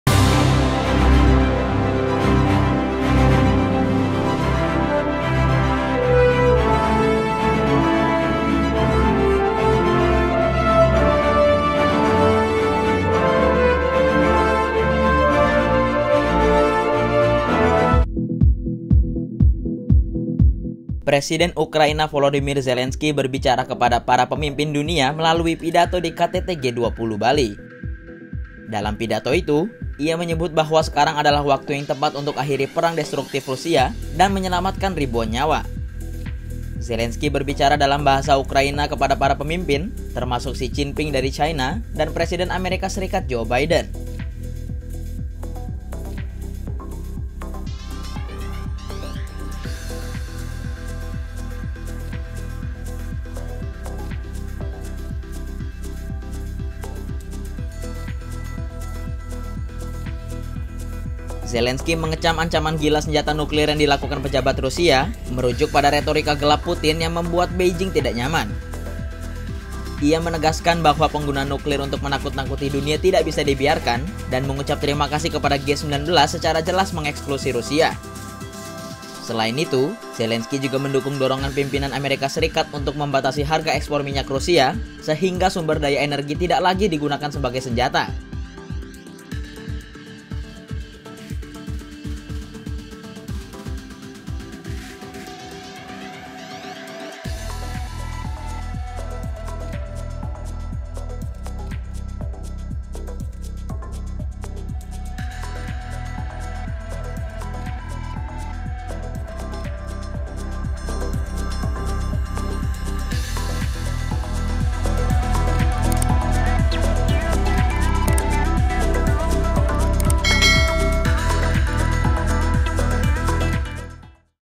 Zelensky Bicara di KTT G20 soal Akhiri Perang Rusia
Presiden Ukraina Volodymyr Zelensky berbicara kepada para pemimpin dunia melalui pidato di KTT G20 Bali.

Dalam pidato itu, ia menyebut bahwa sekarang adalah waktu yang tepat untuk akhiri perang destruktif Rusia dan menyelamatkan ribuan nyawa.
Zelensky mengecam ancaman gila senjata nuklir yang dilakukan pejabat Rusia, merujuk pada retorika gelap Putin yang membuat Beijing tidak nyaman.

Selain itu, Zelensky juga mendukung dorongan pimpinan AS untuk membatasi harga ekspor minyak Rusia, sehingga sumber daya energi tidak lagi digunakan sebagai senjata.

Penulis: Bernadette Aderi Puspaningrum
Penulis Naskah: David Satya Putra
Narator: David Satya Putra
Video Editor: David Satya Putra
Produser: Rose Komala Dewi